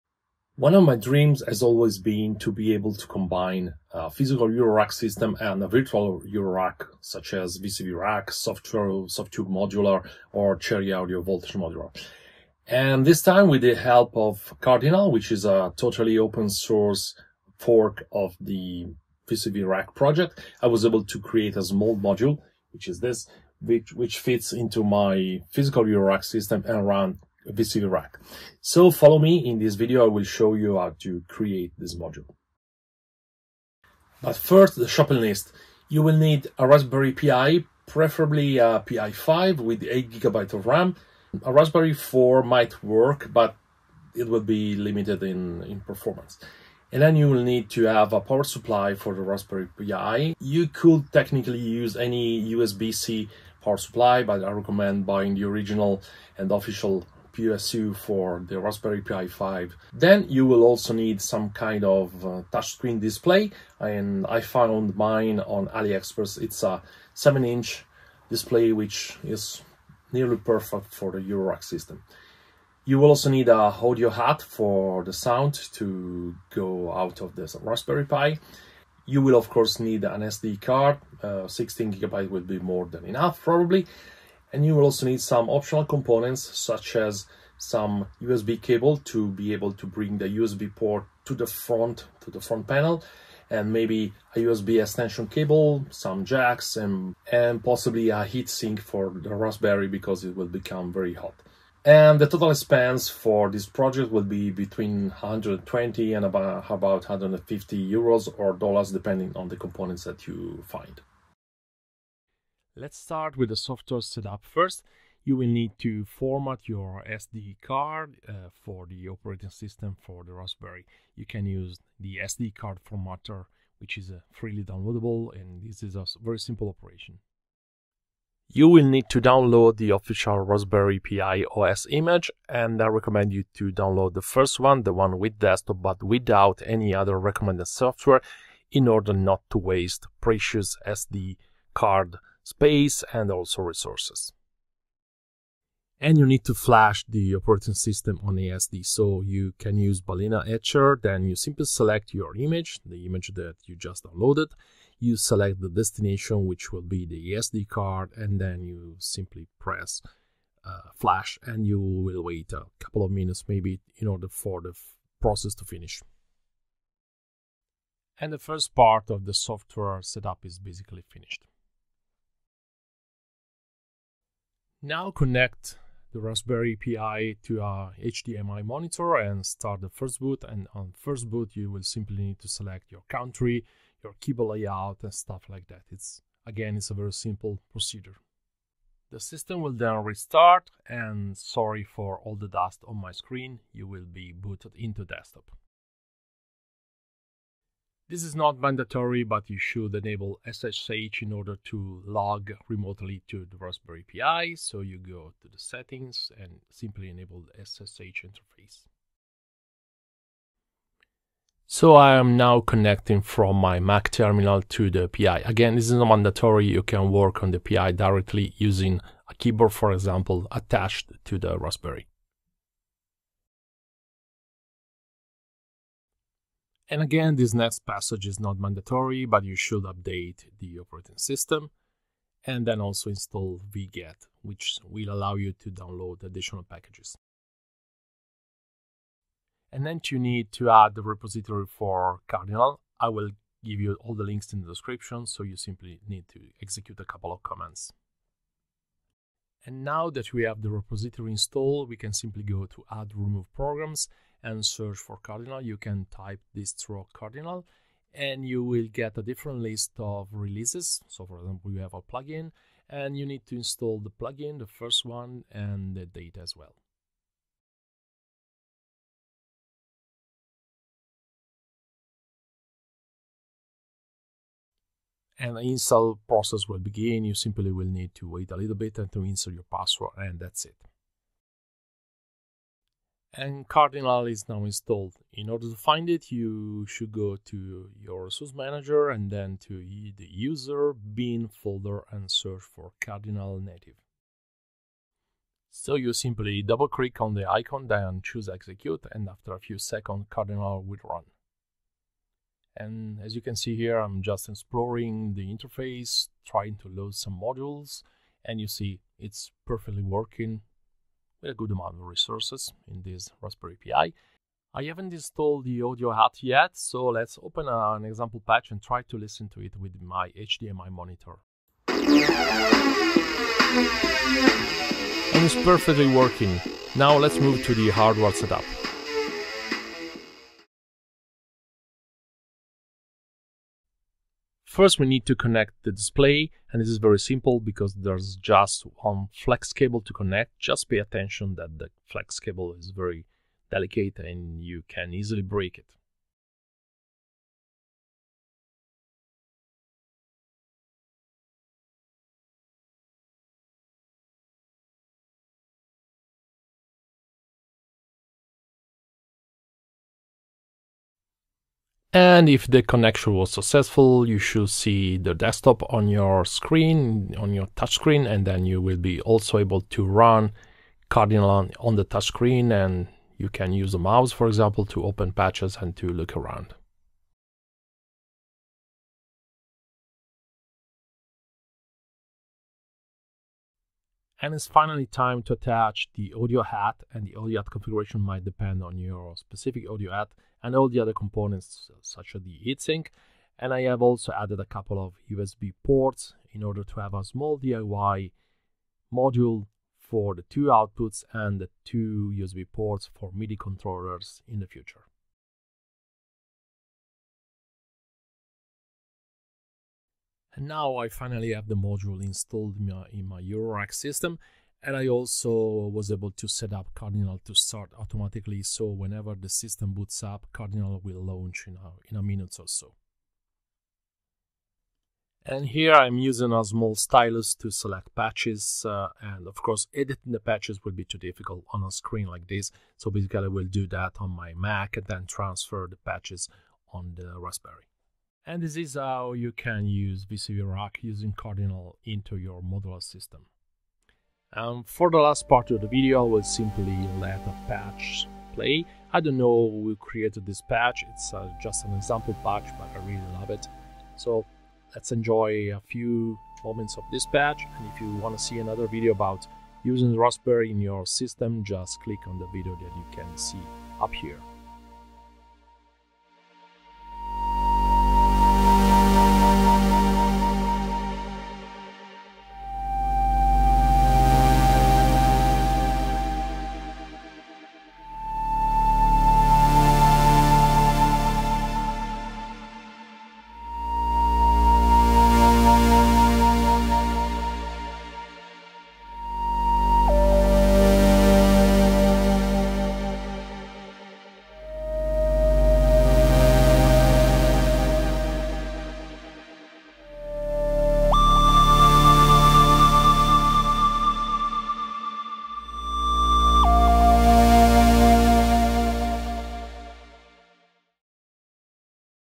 Load VCV Rack in an Eurorack Module | Hybrid Modular Setup
I have built a DIY Eurorack module to load VCV Rack using Cardinal, the Open-Source Fork of VCV Rack.

00:00 introduction
01:52 Software Setup
05:30 Hardware Setup

Raspberry PI 5:

Power Supply:

Audio Hat:

SD Card:

Usb Panel Cables:

USB-C Extension Cable